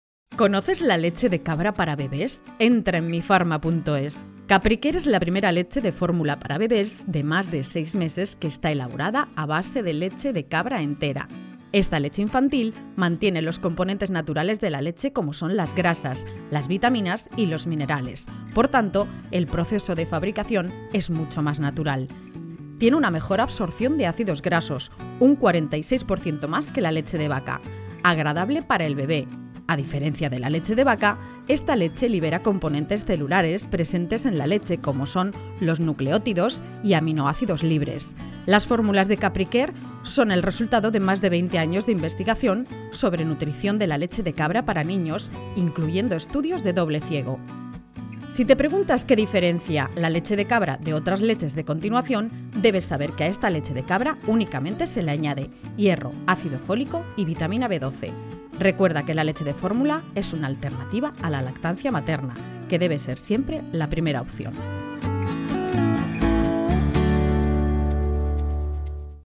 👶🏼🐐 CAPRICARE, leche de cabra para bebés 🐐👶🏼
¿Estás buscando una leche de cabra para bebés? Consigue Capricare entrando en 👉🏼

Capricare es la primera fórmula para bebés que está elaborada a base de leche de cabra entera. Esta leche infantil mantiene los componentes naturales de la leche como son las grasas, las vitaminas y los minerales. Por tanto, el proceso de fabricación es mucho más natural.

¡No olvides que estamos aquí para ayudarte! Cuéntanos ⬇